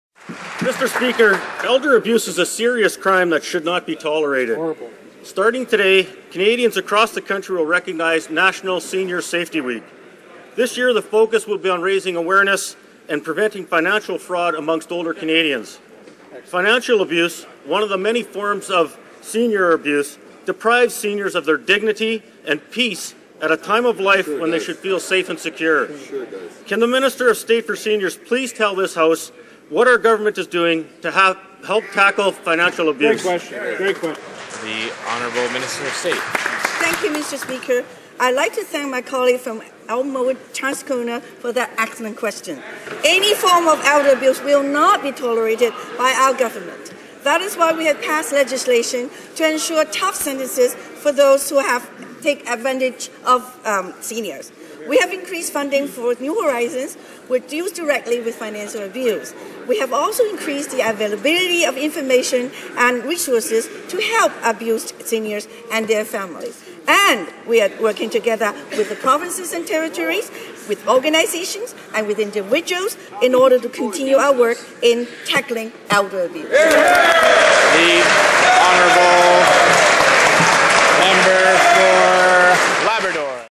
Question - QP November 6, 2013 MP Lawrence Toet (Elmwood - Transcona)
MP Lawrence Toet (Elmwood - Transcona) asks Minister of State for Seniors, Alice Wong, how the Federal Government is addressing senior abuse during National Senior Safety Week.